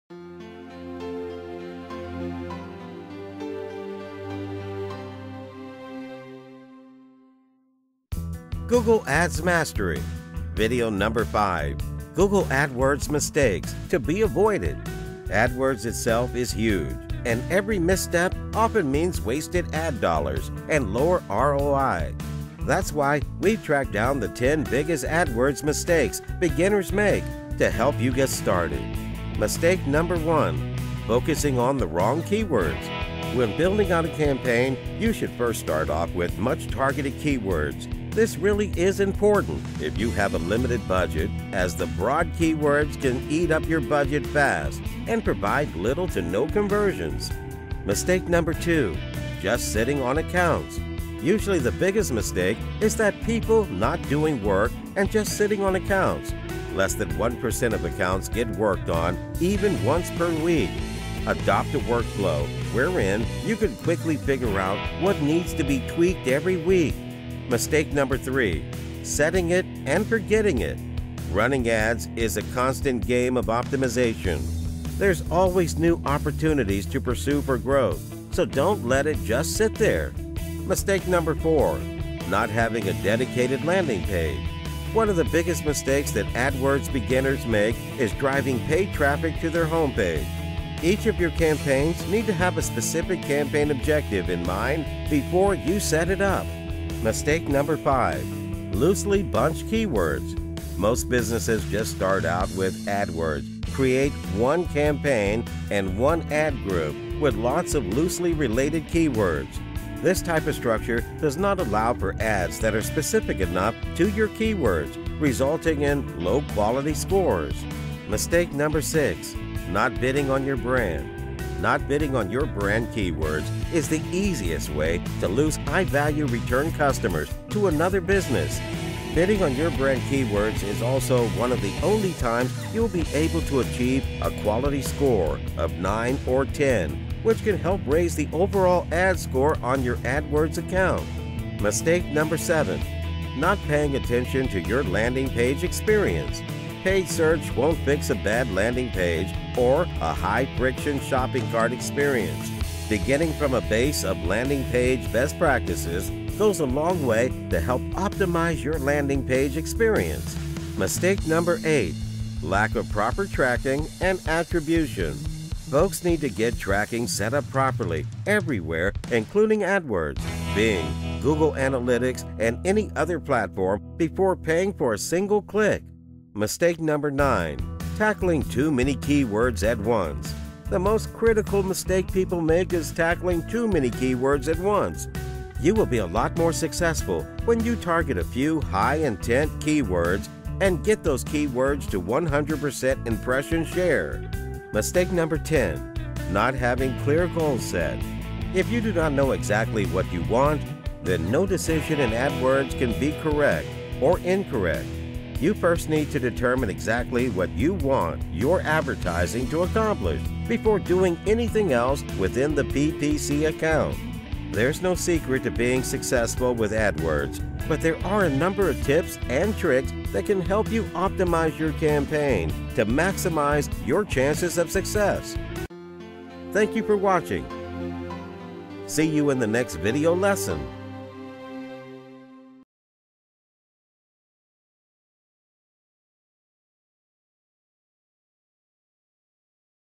Google Adwords Mistakes To Avoid. PH 0468 420 470.
The Google Adwords Mistakes you need to avoid, including the 10 Biggest Adwords Mistakes.  Ring Terry on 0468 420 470. We walk you through Cutting Adwords Costs, setting up Google Adwords Keywords, running Google Adwords campaigns, how to set up your Google Adwords Landing Pages. Learn how to choose the right keywords for Google Adwords, How many Keywords to set up for Google Adwords and setting Goals for Google Adwords.

Good for searches on:
Google Adwords Mistakes to Avoid
10 Biggest Adwords Mistakes